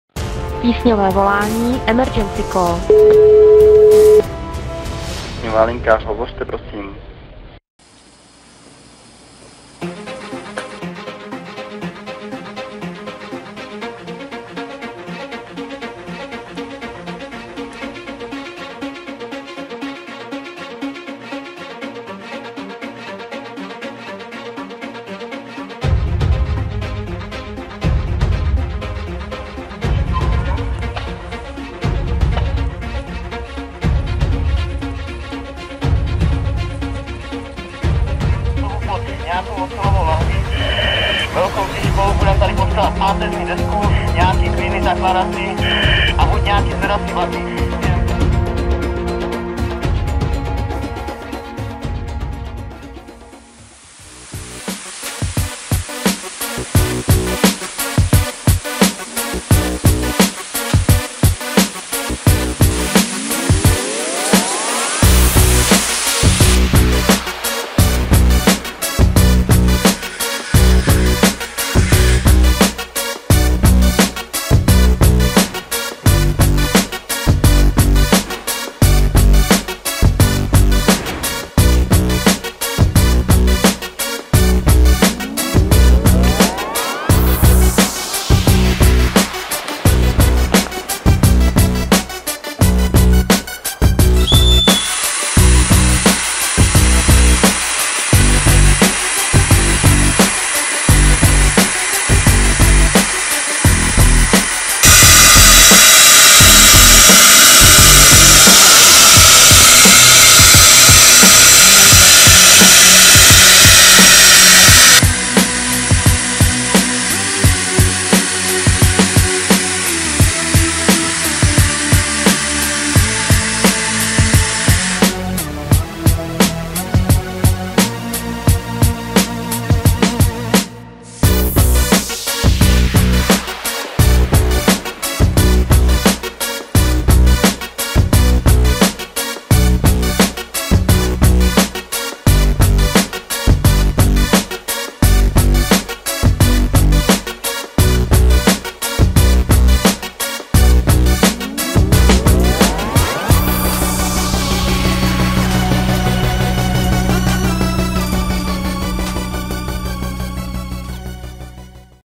Protichemické obleky
Video o ošetřování protichemických obleků po zásahu.